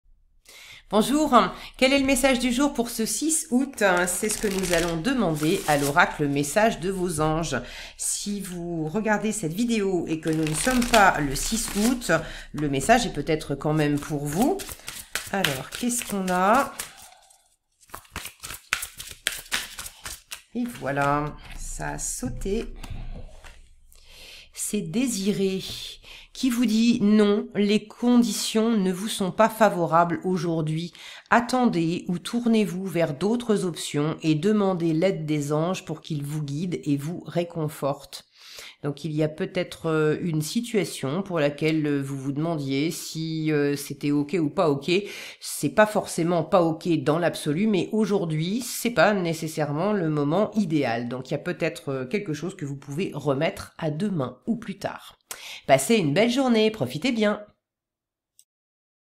Message du jour - 6 Août
👉 Oracle utilisé :
MESSAGES DE VOS ANGES

👉 Prenez uniquement ce qui résonne pour vous, et laissez le reste de côté ! Gardez à l'esprit que rien n'est jamais déterminé et que vous seul créez votre vie avec votre libre arbitre :)

Bon visionnage !